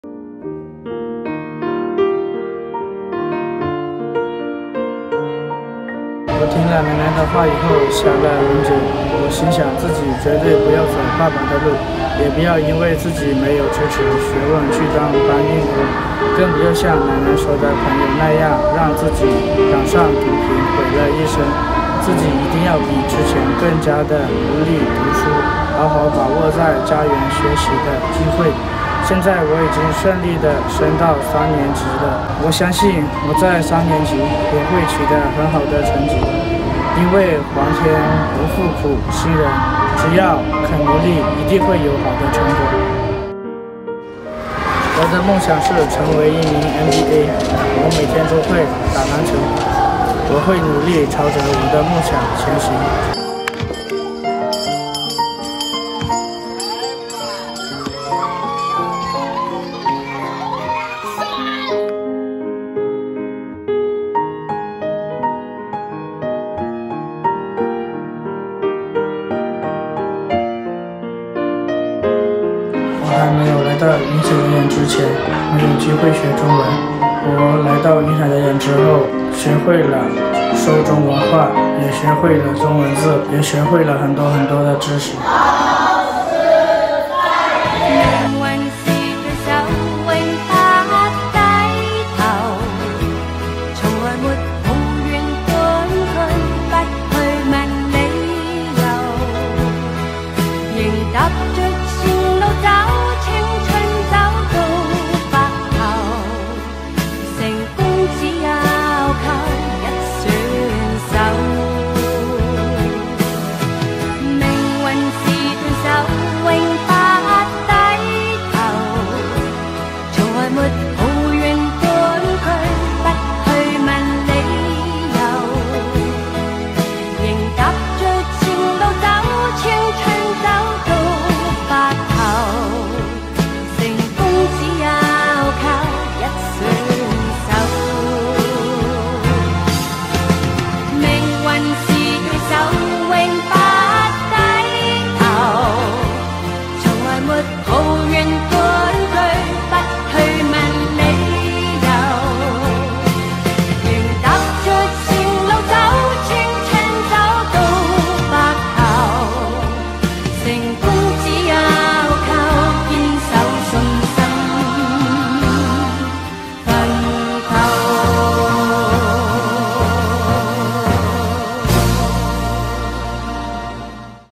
【緬甸孩子生命故事】家雄
我信憑著自己的信念，可以改變自己的命運。
雲彩家園讓他有了自己的目標，一步一步邁向自己不一樣的人生